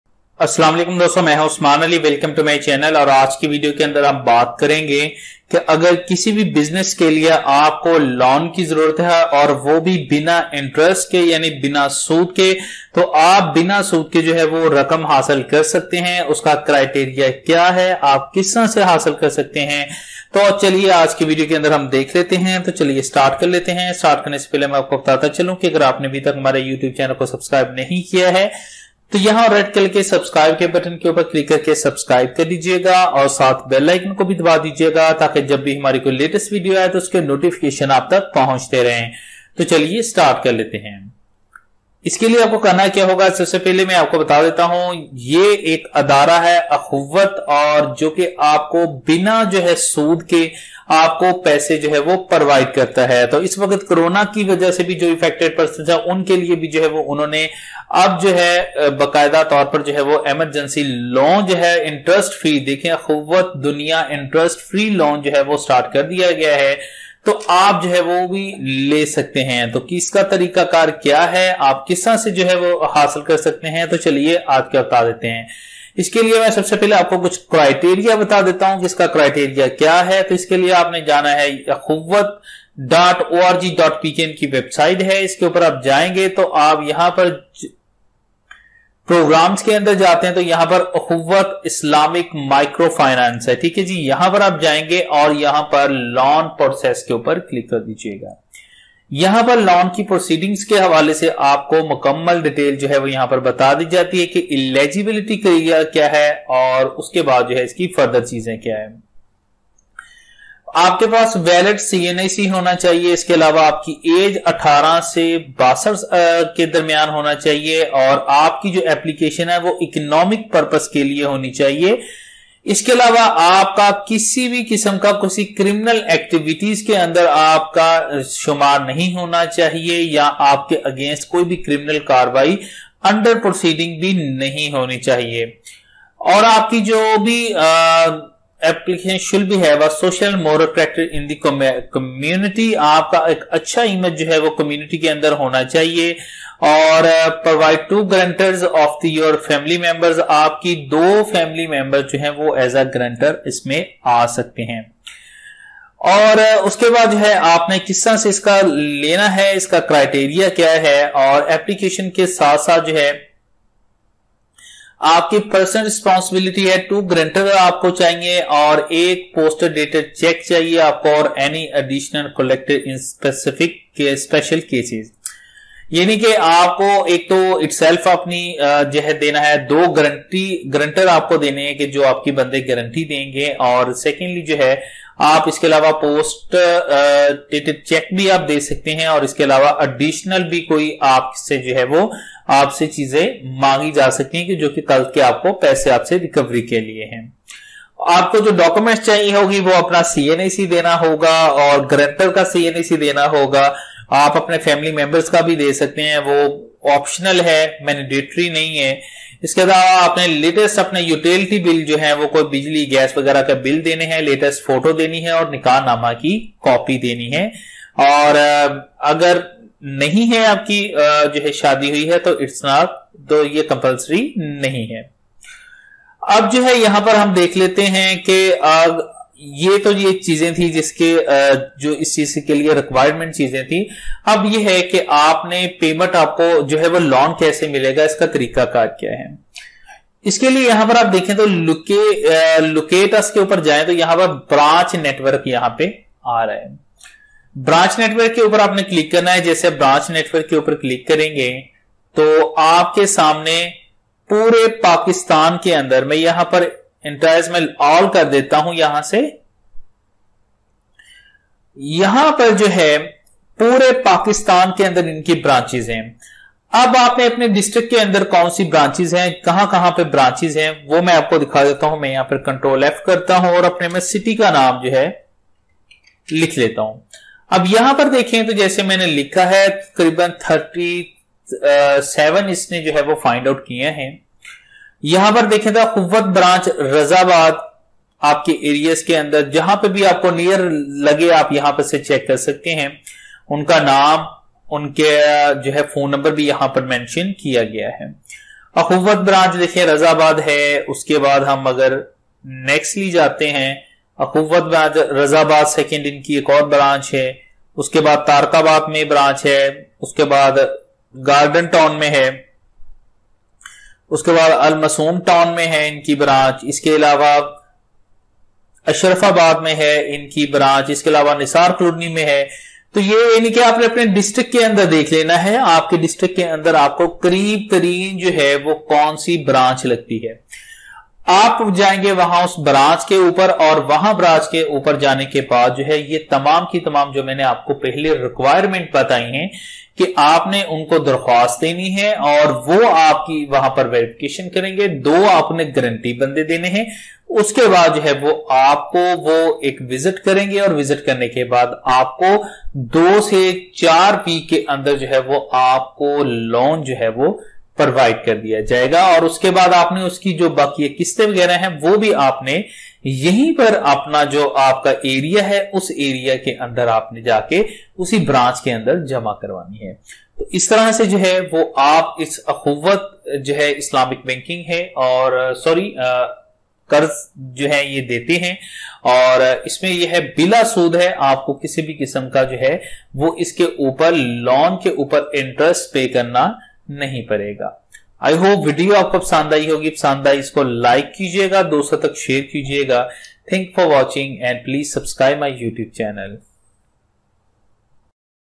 Get Personal Loan without Interest in Pakistan from Akhuwat Micro Finance
Akhuwat is a largest giving loan private department which give a free loan without any interest. In this video tutorial i will gonna show you how to get free loan without any interest in Pakistan with Akhuwat Micro Finance as well as discuss eligibility and required documents.